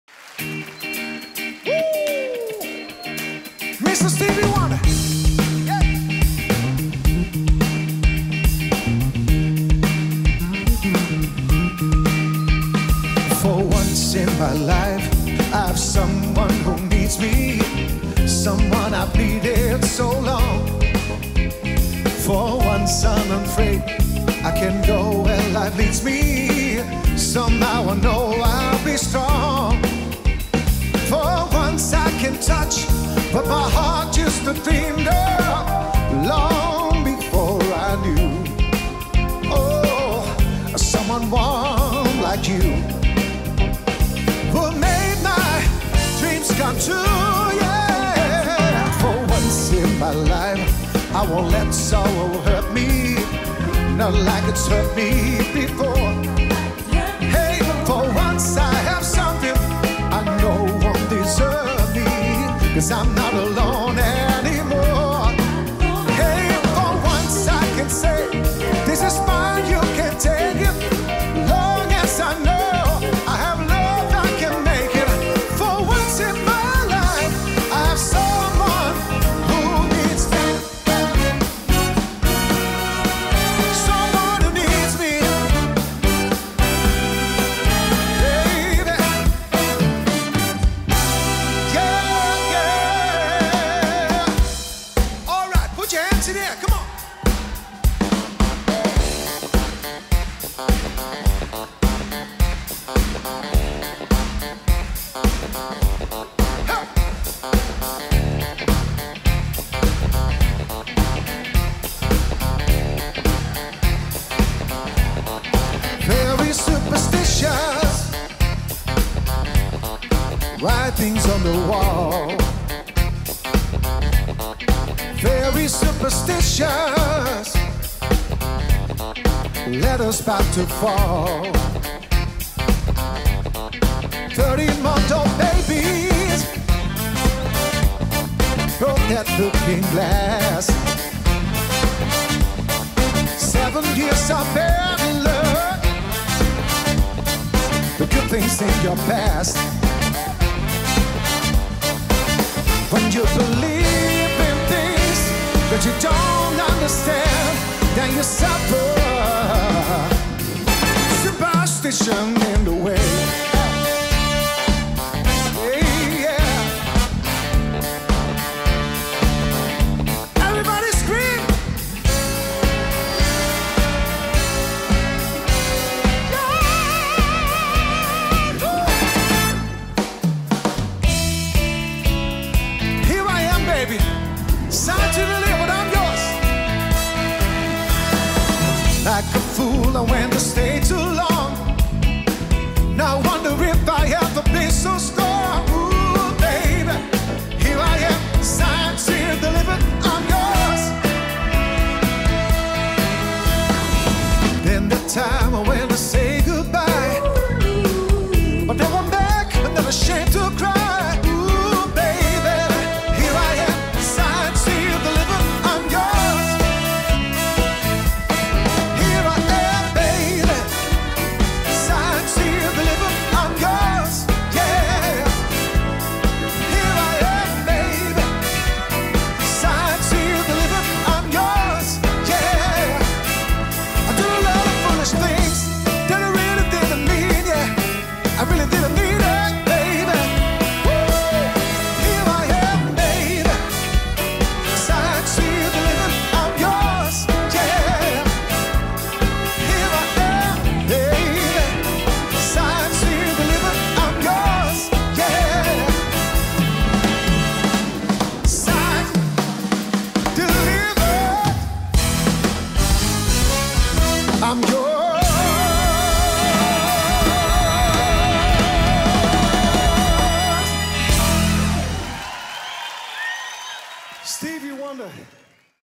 Mike Peterson - Stevie Wonder Medley // Sterren in Concert
Registratie van het concert dat Mike Peterson op 9 december 2019 gaf in het AFAS Circustheater in Scheveningen. Onder begeleiding van een live band zingt hij zijn mooiste liedjes en verrassende medleys. Ook Ruth Jacott komt langs voor een prachtig duet.